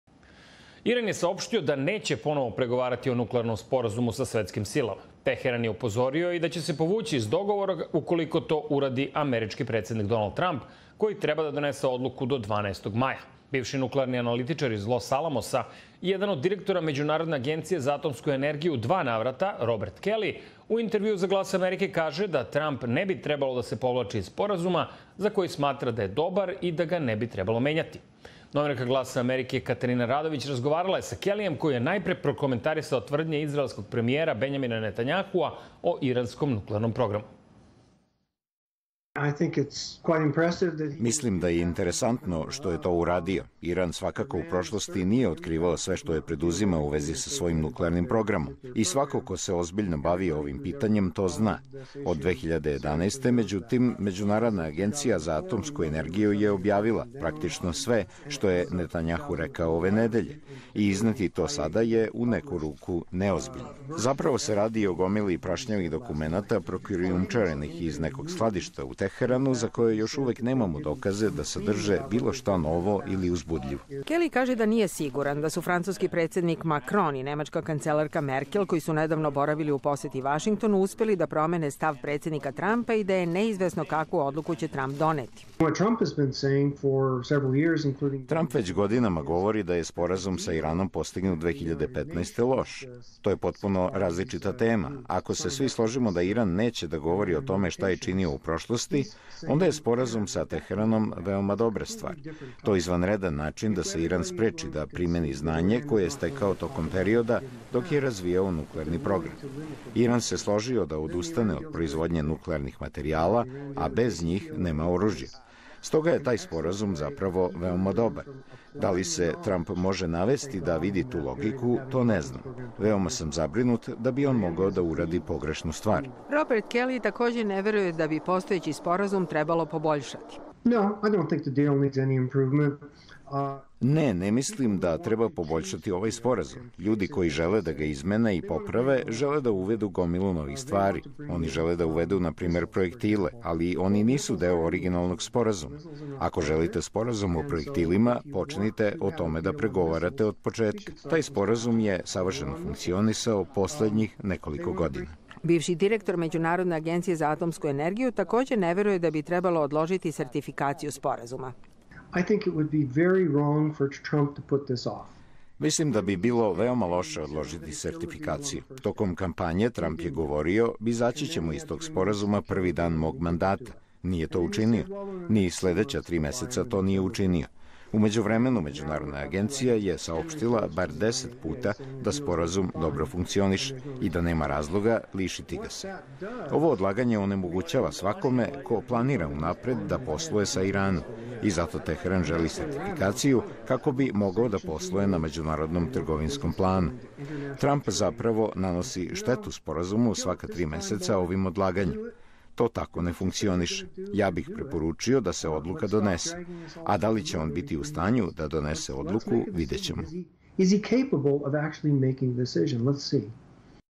Robert Keli: Veoma važno očuvati sporazum sa Iranom
Robert Keli, bivši nuklearni analitičar iz Los Alamosa, koji je u dva navrata bio jedan od direktora Međunarodne agencije za atomsku energiju (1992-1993. i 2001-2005), govori za Glas Amerike o predstojećoj odluci predsednika Trampa da sertifikuje ili ne nuklearni sporazum sa Iranom.